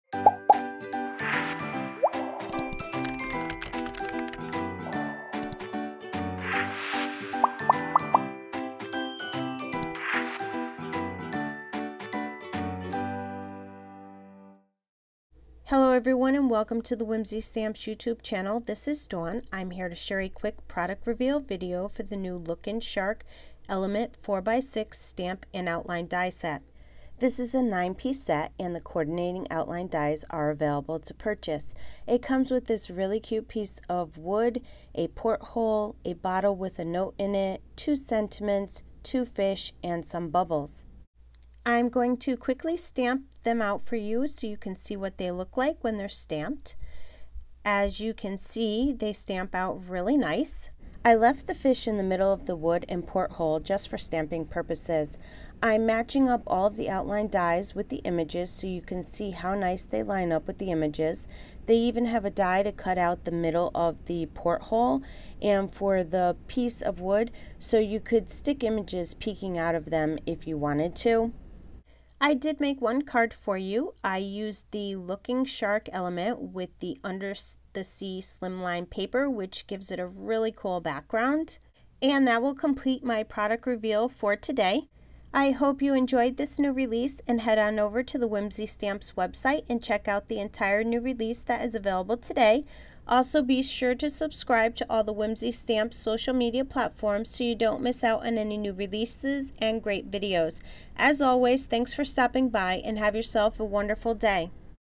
WHIMSY STAMPS PRODUCT REVEAL: Lookin Shark Element Stamp and Outline Die
Hi there! Dawn Here. I have a really great new product release for you. :) The new Lookin Shark Element Stamp and Outline Die.

——— Whimsy Stamps New Release ———

• **NEW Lookin' Shark Elements Outlines Die Set | Whimsy Stamps